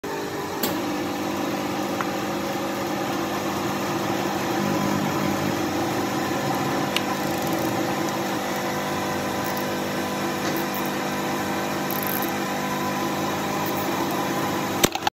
Hydraulic machine vs small bottle of plastic
Have done an experiment with a plastic bottle to see wether it can withstand the original shape or not.